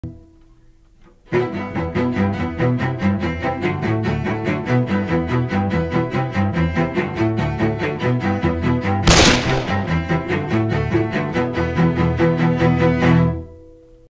[ scrap ] Flat Tire
I was screwing around with the L4D2 rigs this morning. didnt' really do anything with them though. animations janky and coach's butt lights weird I might go back and finsh this one, I wrote down the story at least. but I dont want to take the time away from Wrong Floor at this point. As you can see most of the work just went into coach lifting the car up.